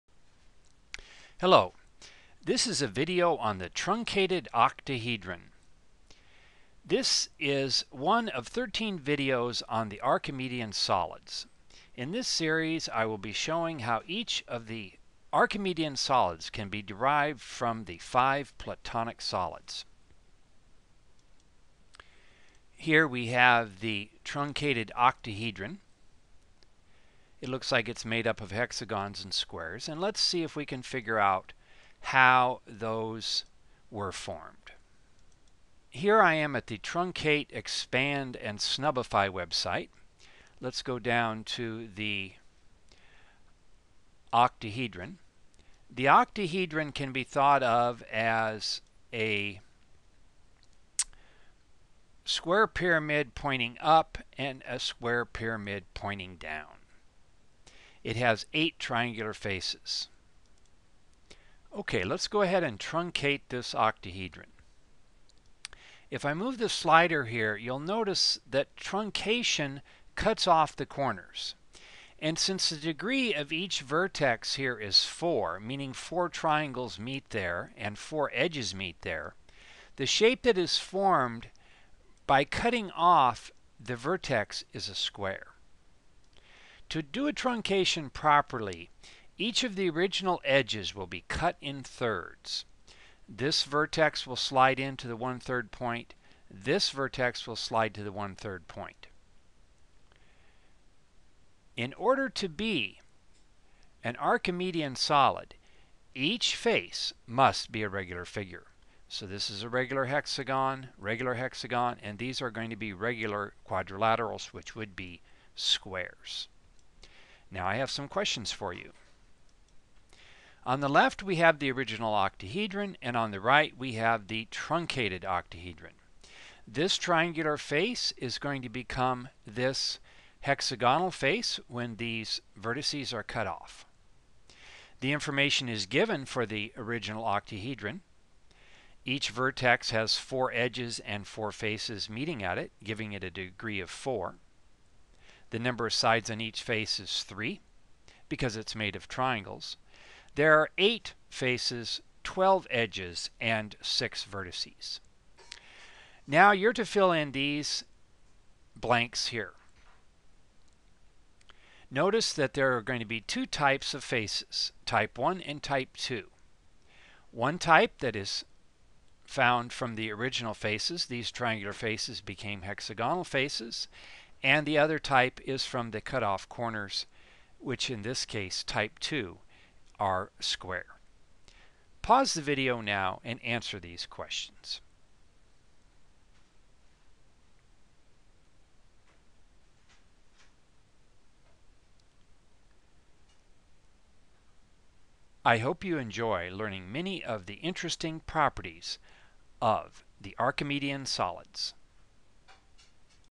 Truncated Octahedron
Explanation of the Truncated Octahedron.  One of 13 videos in the Archimedean Solids series (which can be viewed in any order).  Contains questions to answer at the end.